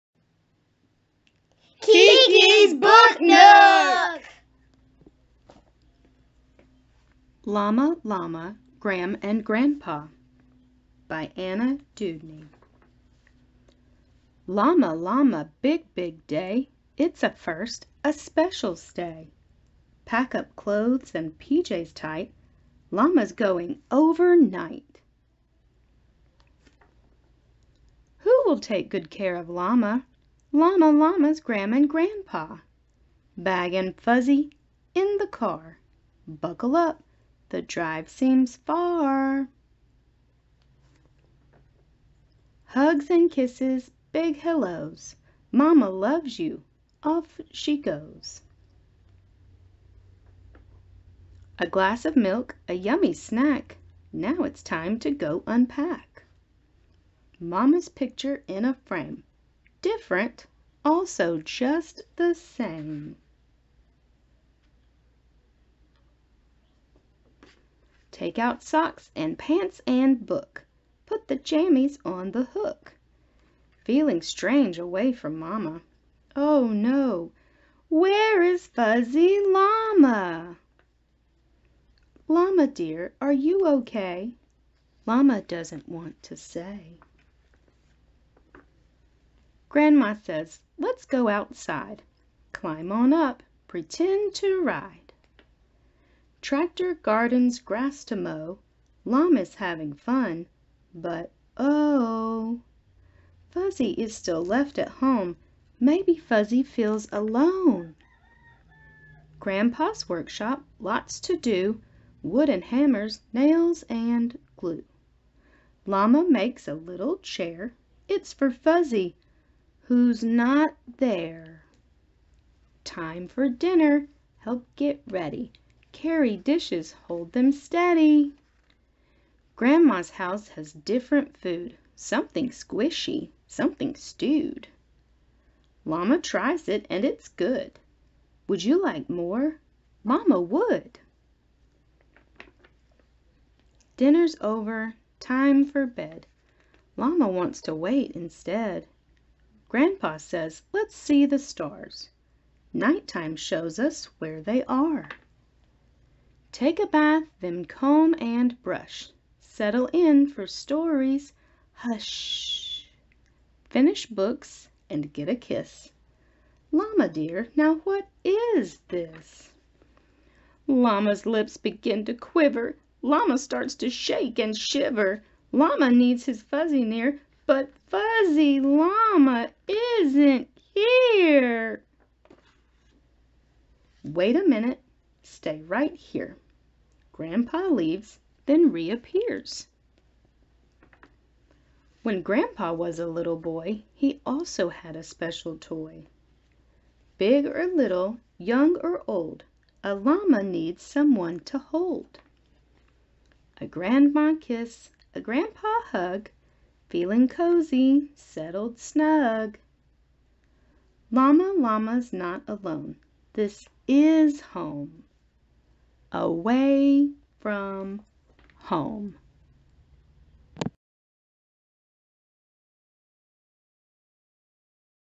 Llama Llama Gram and Grandpa | Read Along | Read Aloud | Children's Book
Llama Llama Gram and Grandpa was written by Anna Dewdney. Sometimes it can be scary staying at a new place, even if it's with grandparents. We often need specific things and get sad if we don't have them. Llama Llama stays the night at Gram and Grandpa's house where some things are different! This is a straight read with no varying voices. It is a fun rhyming book for early readers and pre-readers.

Support the author! Grab a copy and read along!!!